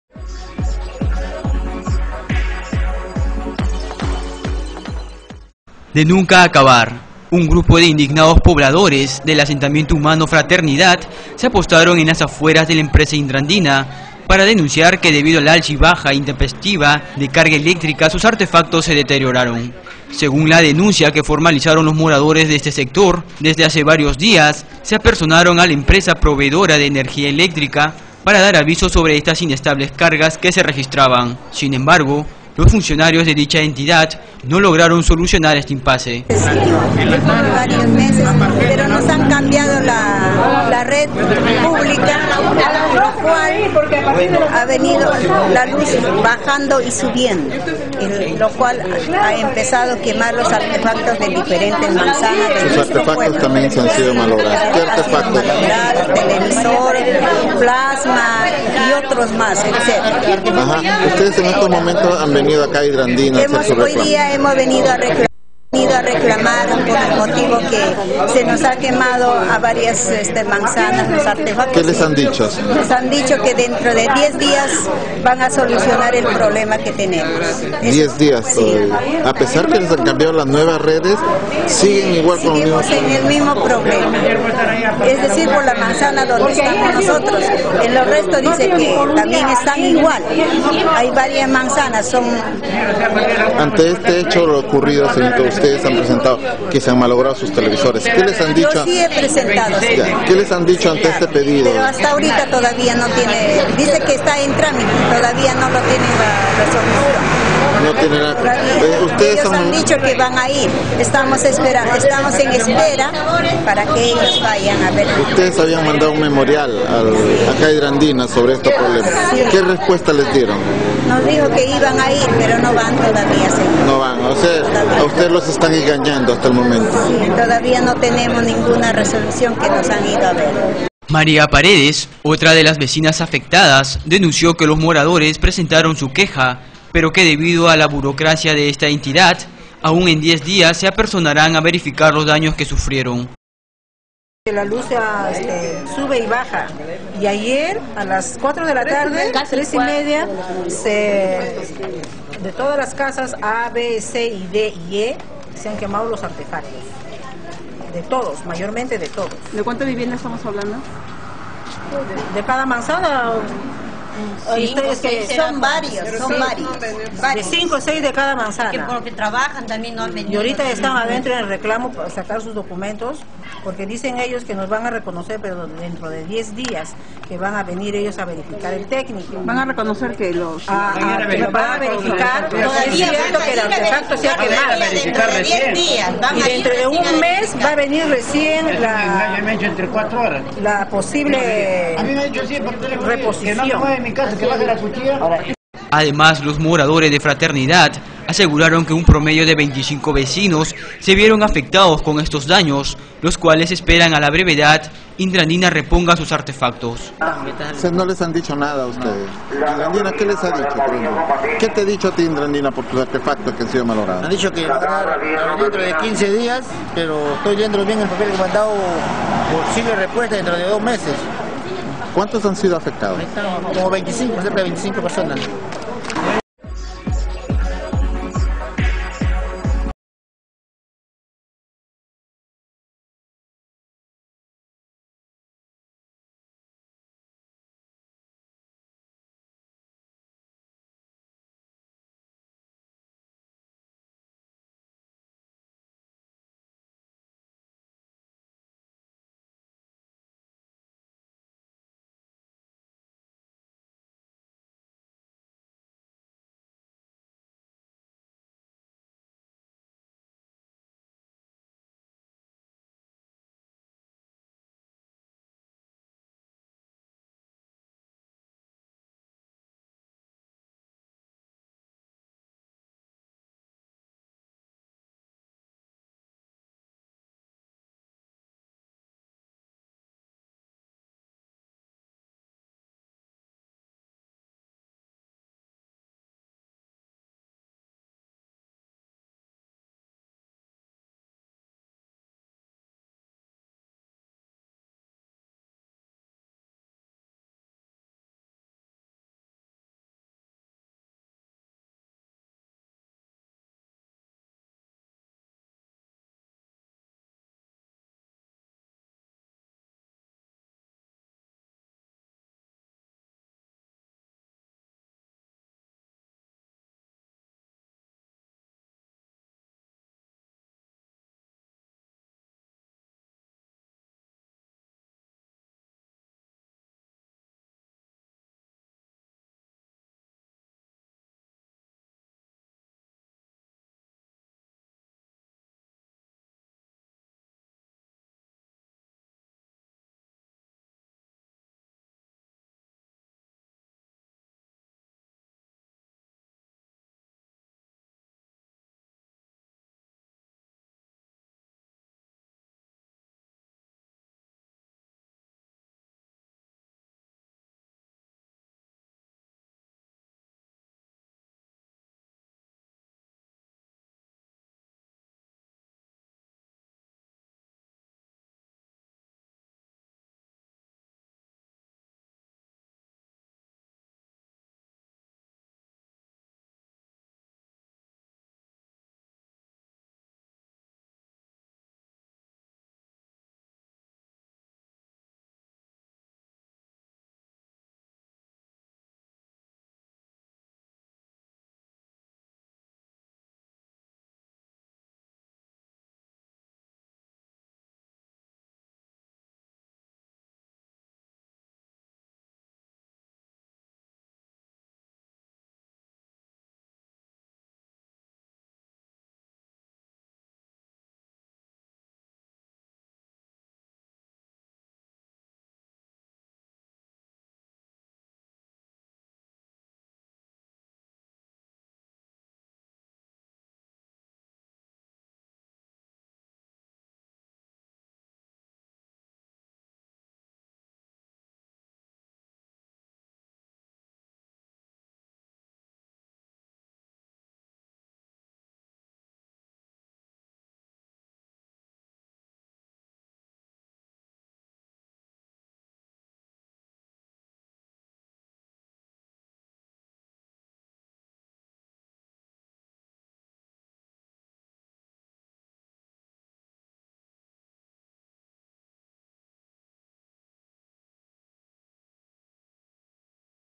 SIN SOLUCION. VECINOS DENUNCIAN A HIDRANDINA POR CARGAS ELECTRICAS INESTABLES
De nunca acabar. Un grupo de indignados pobladores del asentamiento humano Fraternidad se apostaron en las afueras de la empresa Hidrandina para denunciar que debido al alza y baja intempestiva de carga eléctrica sus artefactos se deterioraron.
Según la denuncia que formalizaron los moradores de este sector, desde hace varios días se apersonaron a la empresa proveedora de energía eléctrica para dar aviso sobre estas inestables cargas que se registraban. Sin embargo los funcionarios de dicha entidad no lograron solucionar este impace.
María Paredes, otra de las vecinas afectadas denuncio que los moradores presentaron su queja, pero que debido a la burocracia de esta entidad, aun en 10 días se apersonaran a verificar los daños que sufrieron.
Además los moradores de Fraternidad aseguraron que un promedio de 25 vecinos se vieron afectados con estos daños, los cuales esperan a la brevedad Hidrandina reponga sus artefactos.